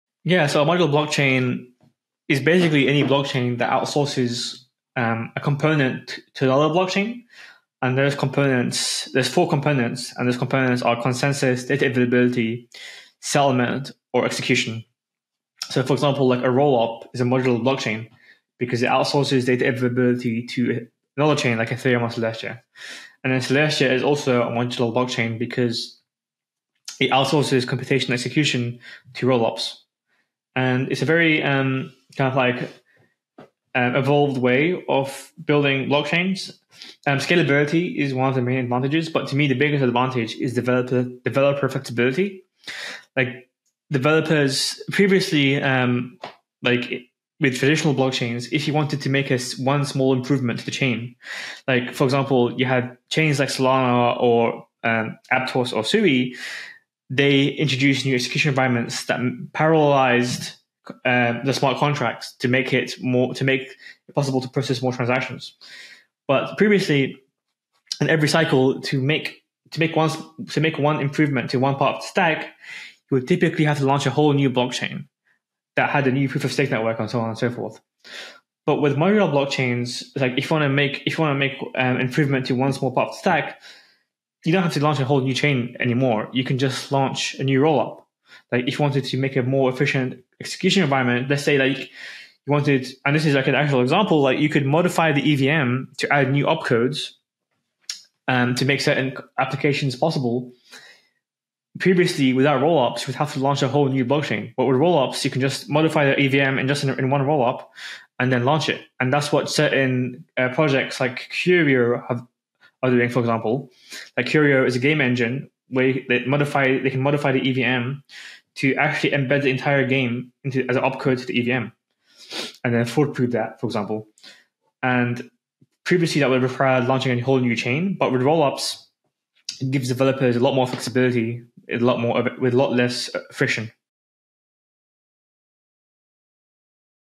What is a Modular Blockchain?
Celestia Co-Founder and CEO Mustafa Al-Bassam describes Modular Blockchains.